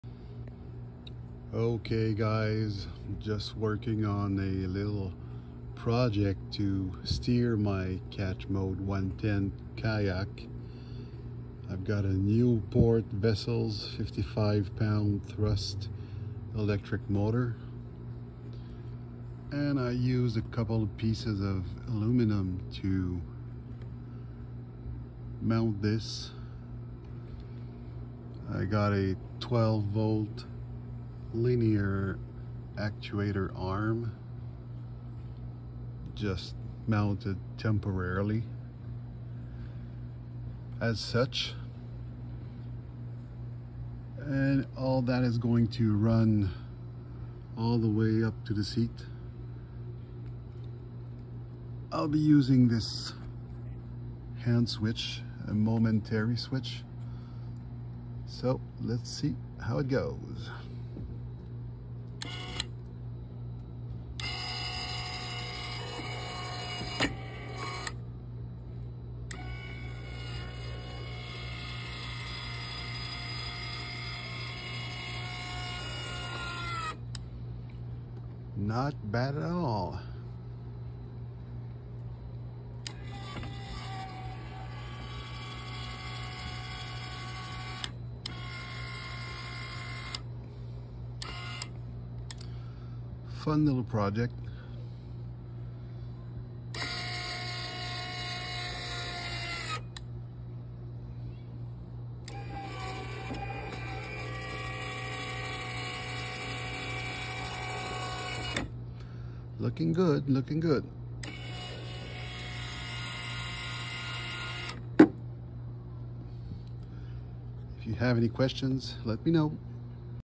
12V linear actuator arm steering a trolling motor on a kayak
Fun little project to add steering capabilities to the trolling motor on my Pelican Catch Mode 110 fishing kayak.

Still a work in progress, but I hope it inspires you to do the same thing.

At this moment, I still need to adjust the speed manually by reaching behind me, but steering the kayak with a remote that sits in the palm of my hand… priceless :)